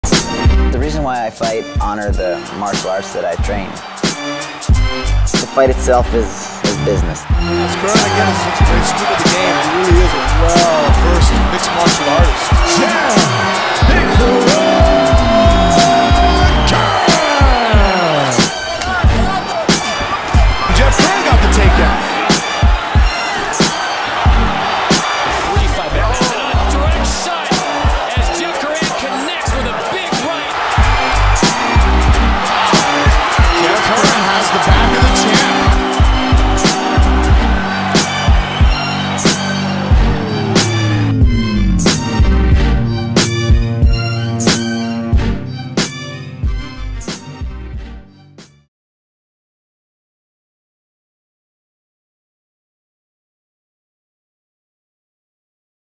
WEC Highlight Video of Jeff Curran
This was a highlight video done on Jeff Curran before he fought Takeya Mizugaki in WEC